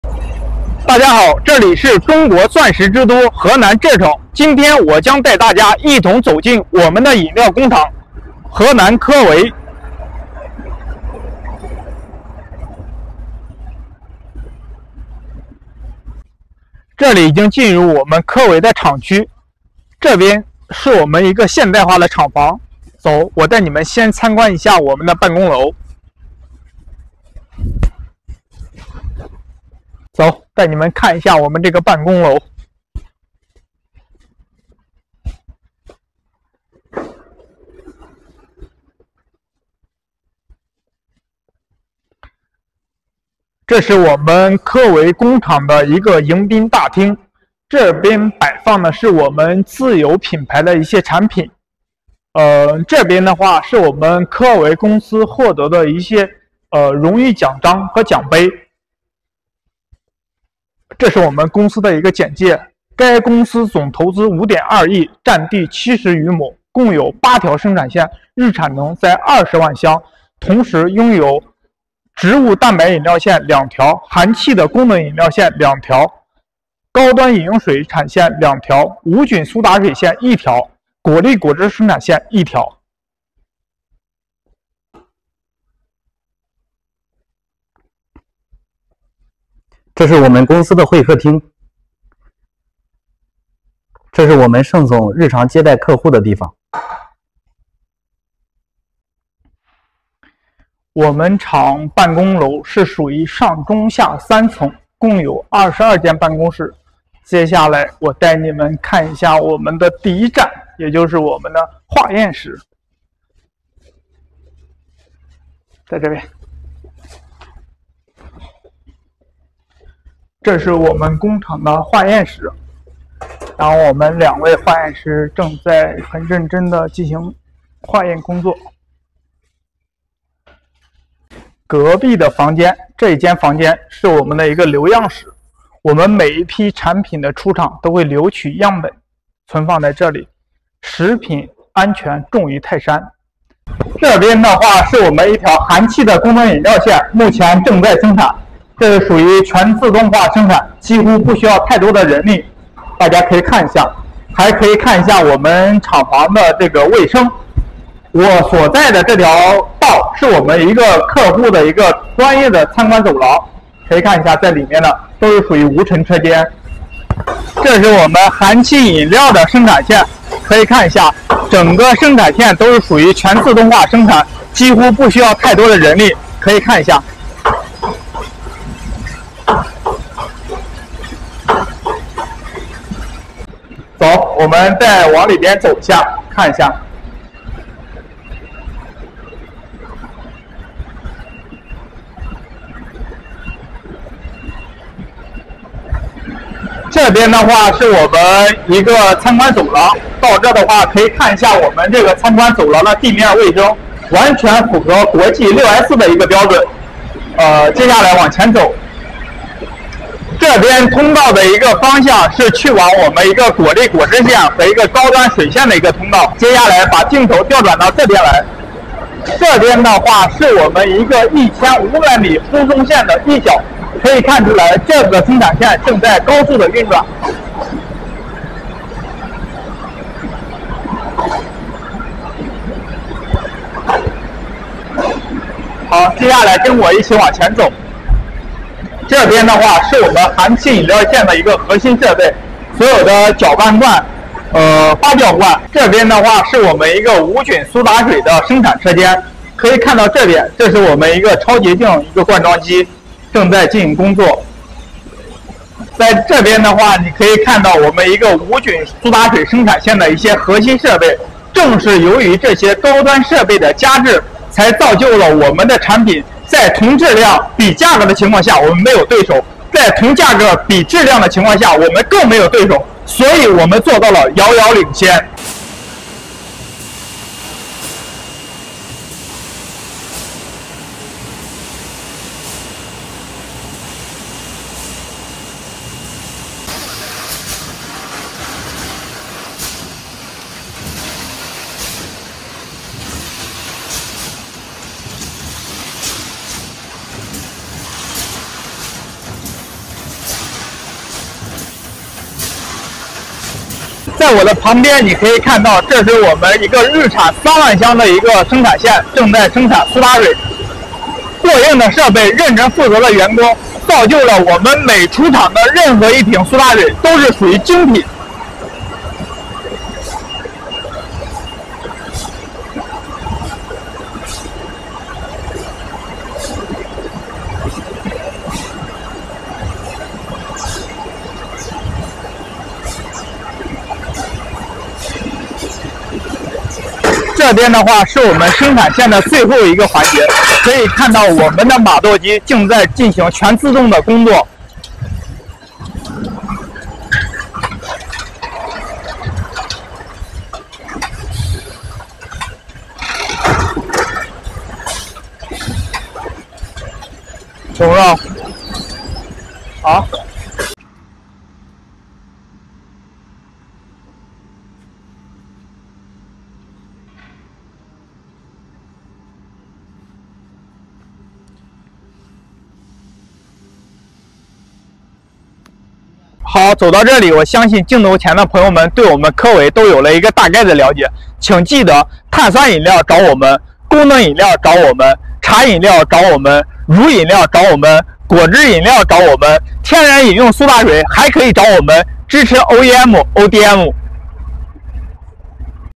Inside Henan Kewei Biological Food Co.: High-Tech Beverage Production || 河南科威生物食品有限公司内部：饮品生产的高科技卓越
ENGLISH: Henan Kewei Biological Food Co., Ltd. is located in Pudong Economic Development Zone, Zhecheng County, east of Henan, covering an area of 50,000 square meters and a construction area of 30,000 square meters. The company has more than 100 employees, more than 30 middle and senior technicians, and 30 bachelor's degrees or above. The company has two intelligent plant protein beverage lines, two gas-containing functional beverage lines, one sterile filling line, three high-end drinking water production lines, and one sterile soda production line. The company focuses on plant protein drinks, The R&D, production and sales of functional drinks, high-end drinking water and soda are high-tech enterprises with integrated R&D, production and sales.

The company has more than 80 leading products and has won the "Best Seller Award for Dealers in China's Food Industry", "Brand Strategic Partner", "Innovative Enterprise", "Most Potential Award" and other honorary titles for many consecutive years.

The company has a first-class R&D team, which is committed to the research and development of new products, and strictly controls the quality from the source to ensure product quality and ensure that every bottle of products into the mouth of consumers is worry-free.

中文：
河南科维生物食品有限公司坐落于豫东，柘城县浦东经济开发区，占地面积5万平方米，建筑面积3万平方米。公司现有职工100余人，中高级技工30余人，本科及以上学历30人，拥有智能化植物蛋白饮料线两条，含气功能饮料线两条，无菌灌装线一条，高端饮用水生产线三条，无菌苏打水生产线一条，公司专注于植物蛋白饮料、功能饮料、高端饮用水及苏打水的研发、生产与销售，是具备研发，生产，销售一体化的高新技术企业。

公司主导产品80多种，连续多年获得“中国食品行业经销商推荐畅销产品奖”，“品牌战略合作商”，“创新型企业”，“最具潜力奖”等荣誉称号。

公司拥有一流的研发团队，致力于新产品的研发，并从源头出发严把质量关，保证产品质量，保证每一瓶进到消费者口中的产品都安心无忧。